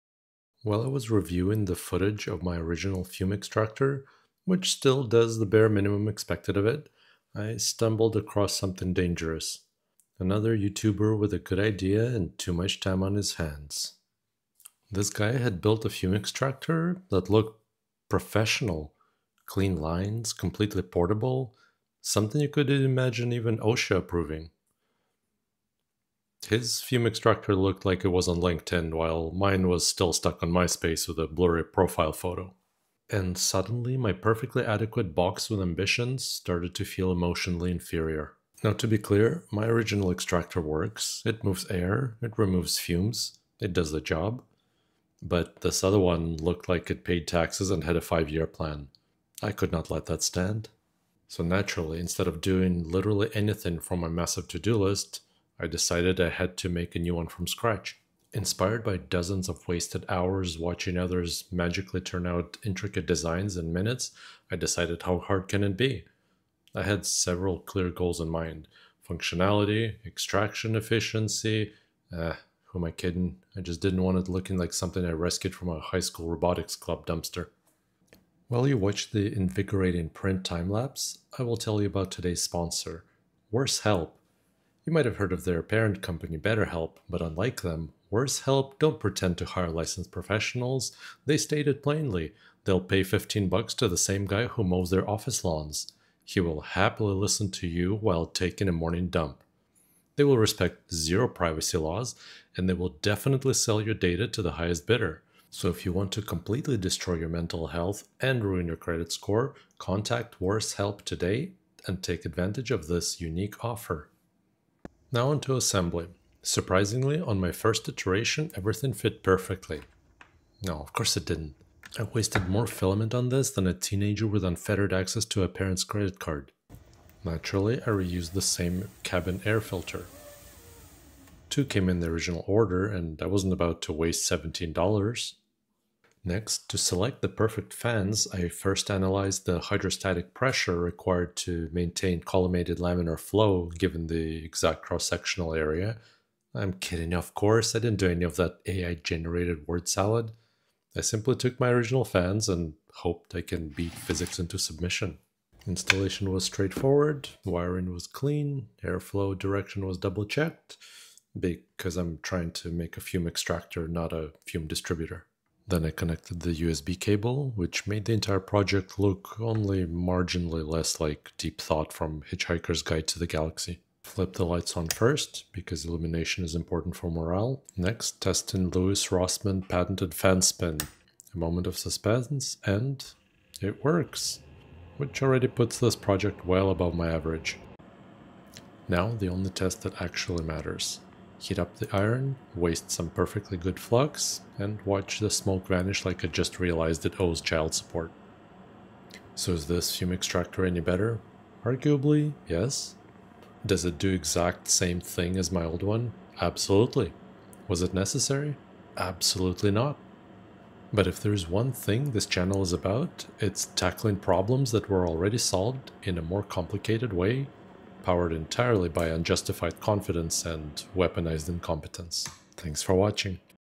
#39 Fume Extractor 2.0 - 3D printed, usb-c powered with lighting
Since making the first fume extractor I had come across and it struck a nerve. I had to have something similar. But double the area, double the fans, double the problems.

Brass Sponge

Fluke 101 Multimeter